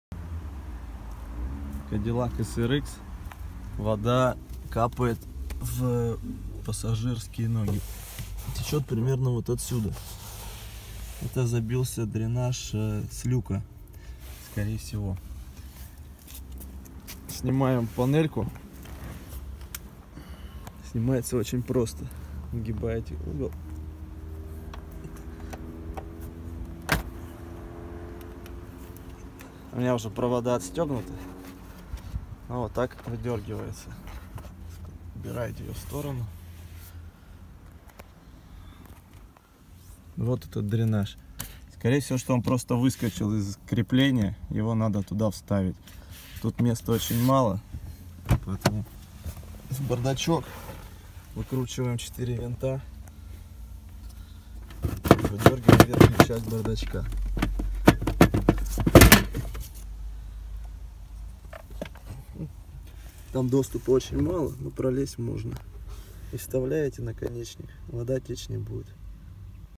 Cadillac SRX вода в салоне пассажирское кресло
после дождя вода в ногах переднего пассажира в кадиллаке срх
слетает шланг дренажа с крепления под торпедой.
ставим шланг на место через бордачек.